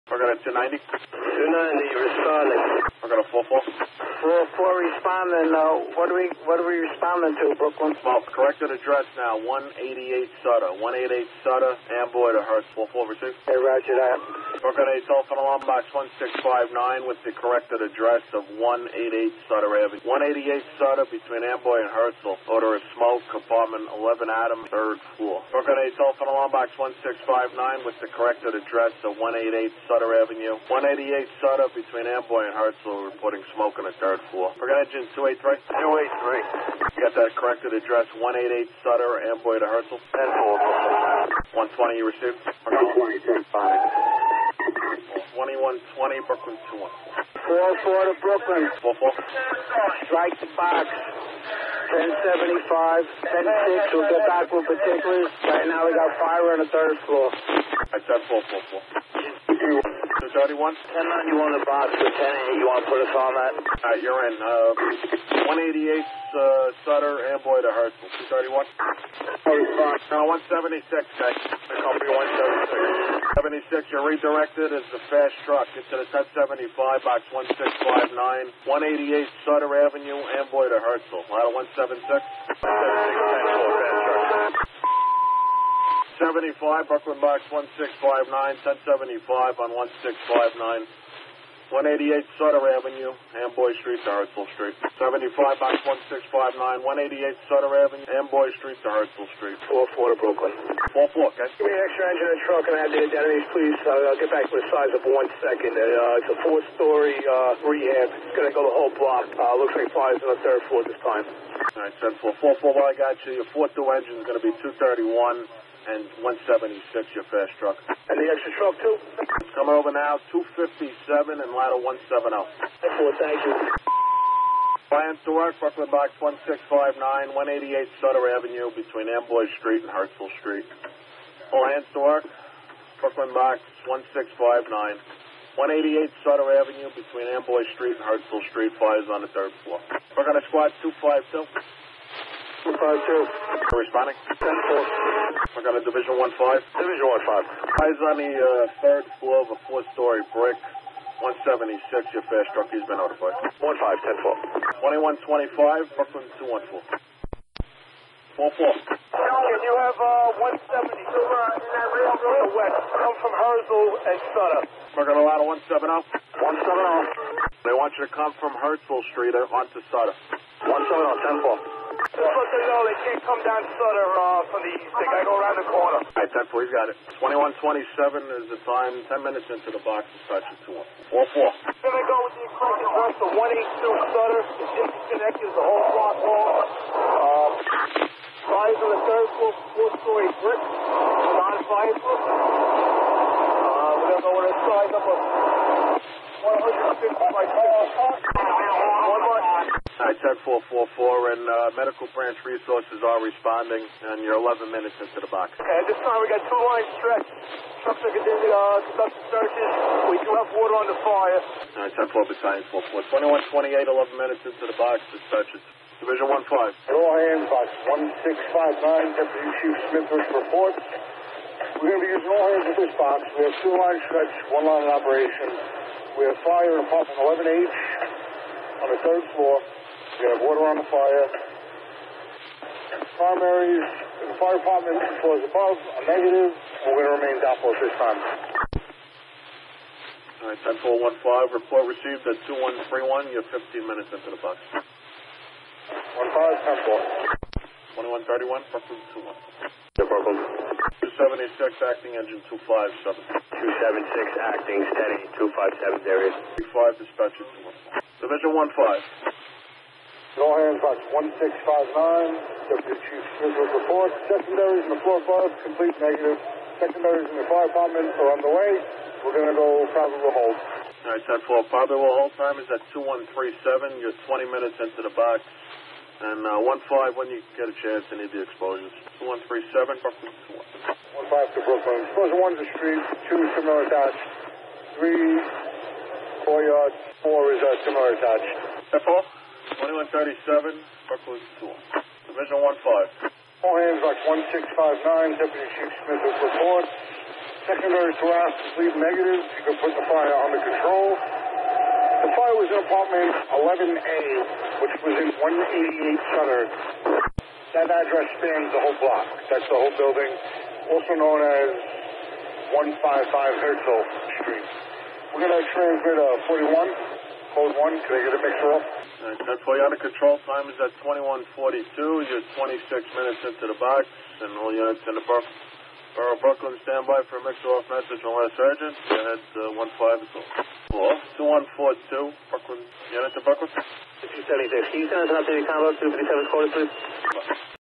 NYCFireAudio - FDNY Brooklyn All Hands Box 1659 Audio - Fire in A Multiple Dwelling - 9/1/18
AUDIO PROVIDED BY BROADCASTIFY

Fire Location: 182 Sutter Ave

B-44 gave the 10-75

Fire on the 3rd floor of a 4 story brick rehab building

L-176 Fast
E-231 4th

B-44 All Hands extra Engine & Truck 257/170

Maybe:*G-Man*
E-290,283,233,231,257s/c
L-120,103,176(Fast),170s/c
B-44,38
R-2
Sq-252
D-15
Rac-2